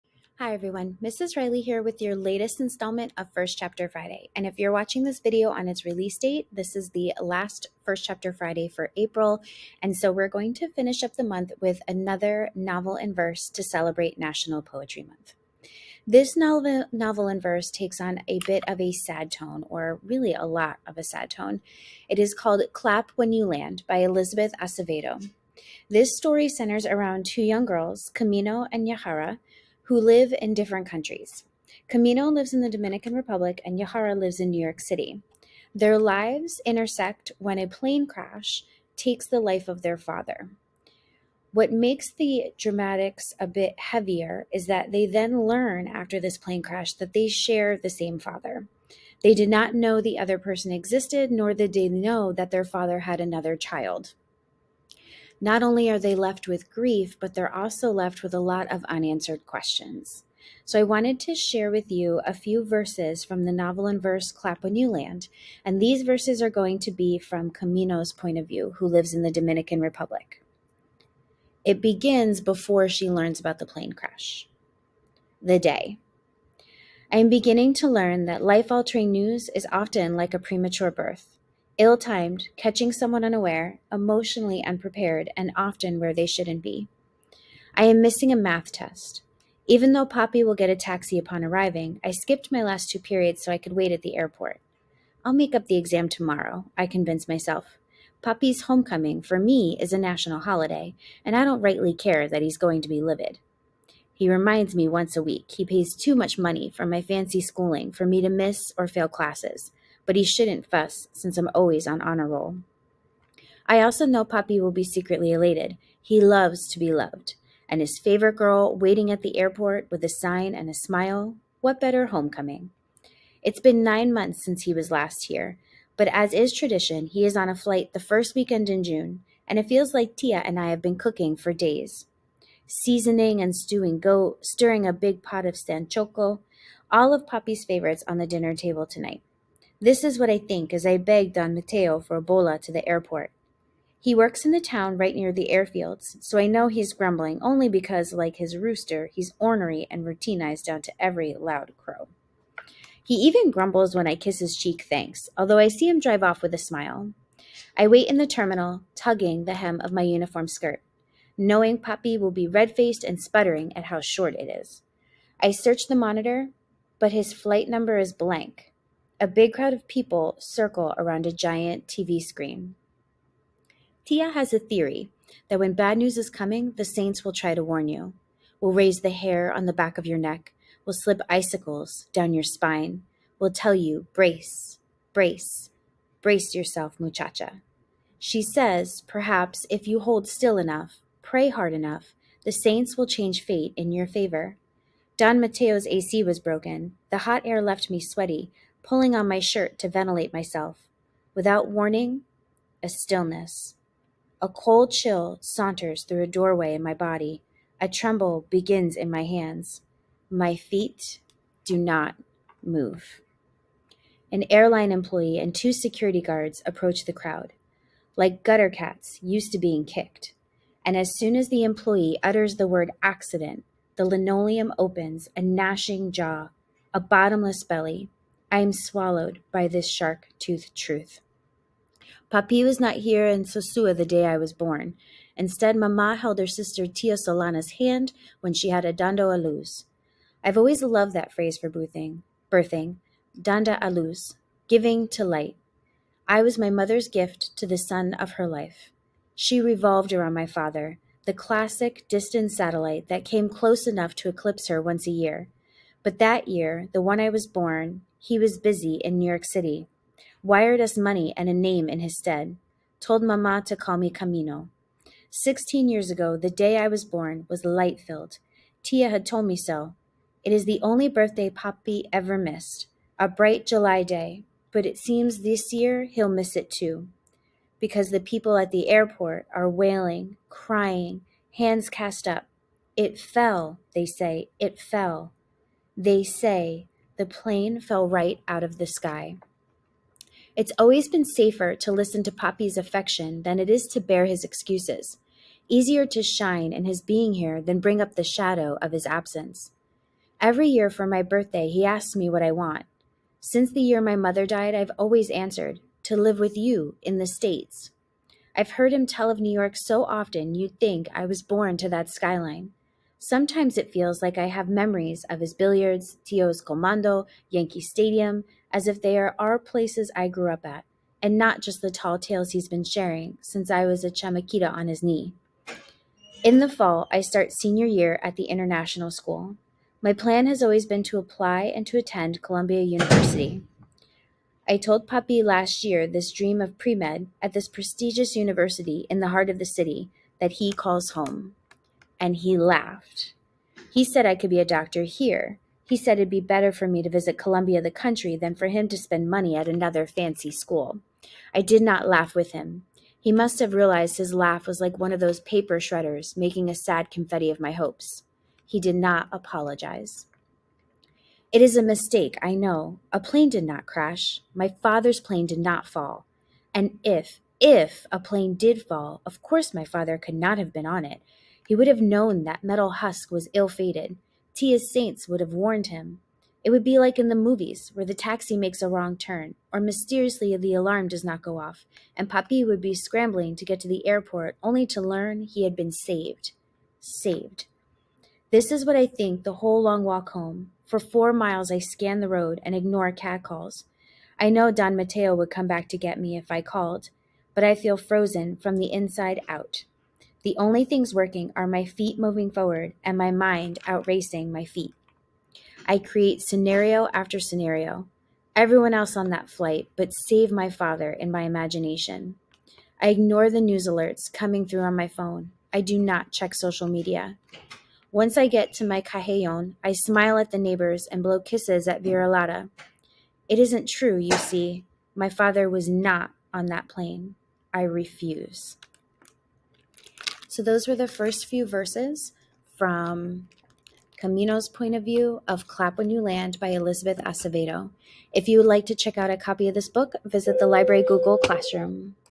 First Chapter Fridays: Clap When You Land by Elizabeth Acevedo
Mrs. Reilly shares one more novel in verse to celebrate National Poetry Month, this time with a few verse from Elizabeth Acevedo's Clap When You Land.